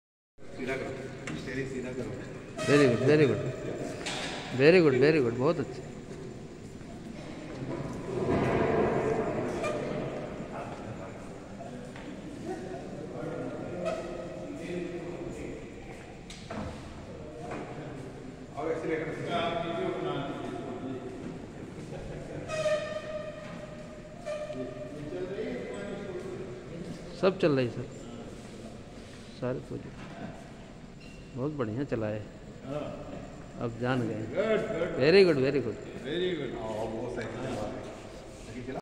Jaitra Singh drove Solar Street Car at SMS on 27 Jan 2024
Solar Street Car is developed under student projects. It was first kind of Car which runs on green energy (i.e., Solar PV) and developed at SMS in the year 2013. It was demonstrated before the Former President of India on 10th May 2013 at BBAU, Lucknow and received recognition amongst 25 projects selected at UP State level by UP CST.